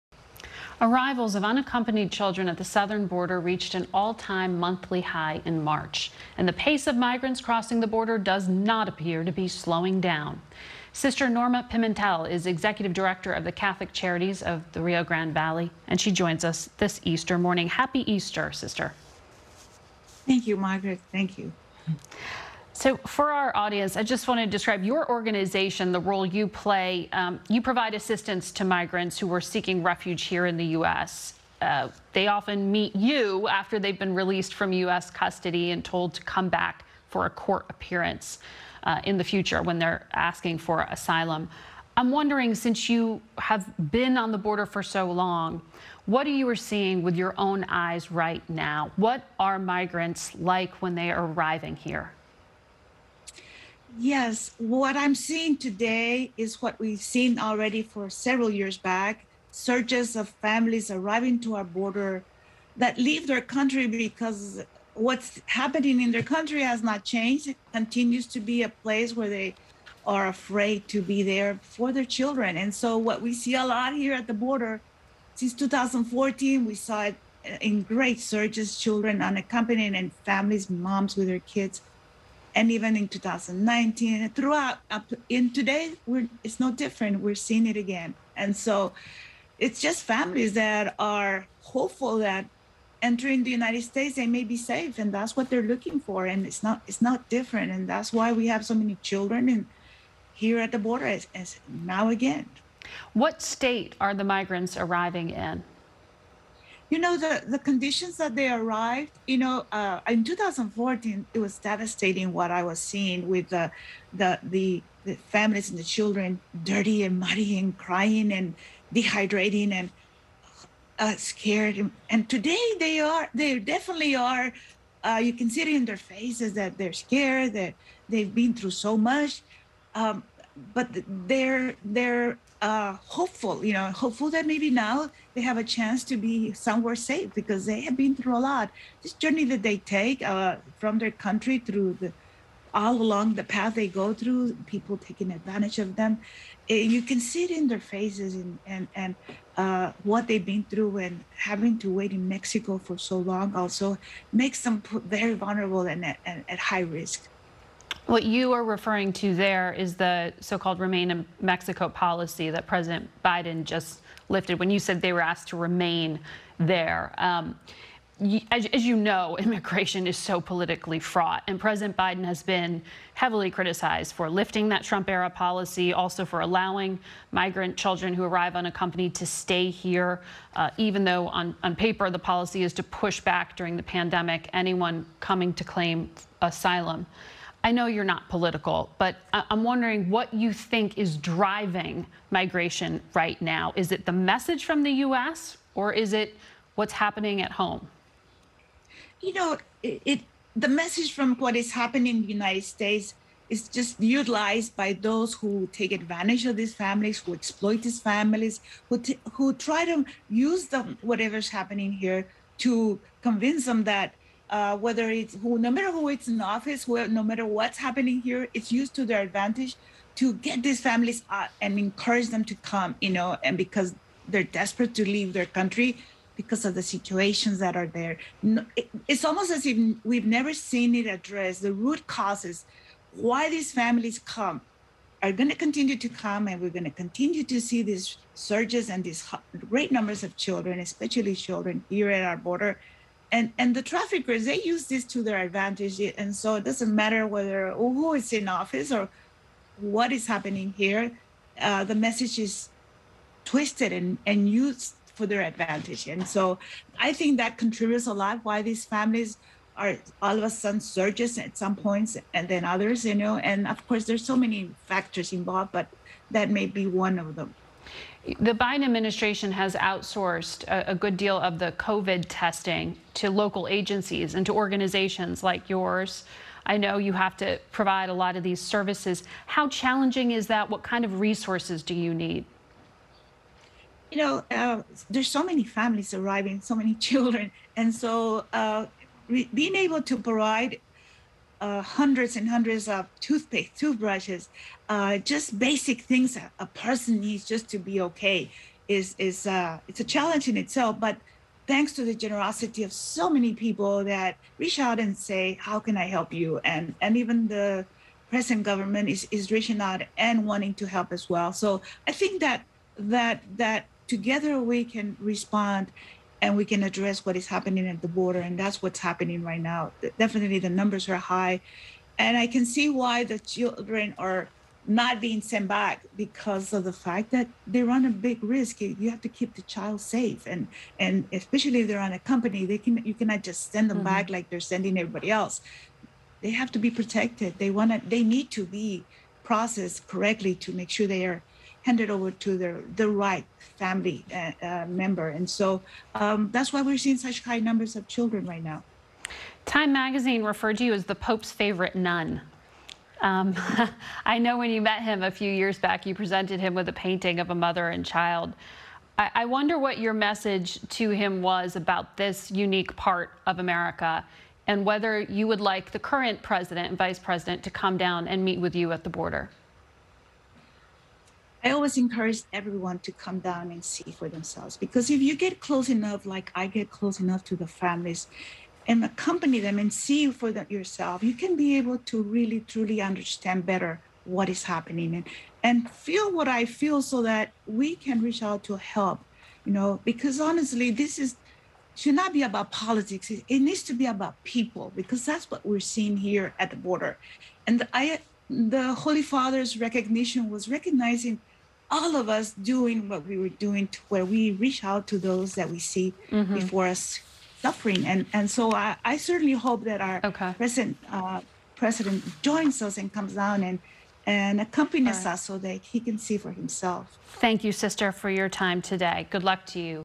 Head of Catholic Charities along border says migrant families are "desperate" to flee home countr…
Sister Norma Pimentel, the executive director of Catholic Charities of the Rio Grande Valley, says families are "hopeful that entering the United States may be safe, and that's what they're looking for."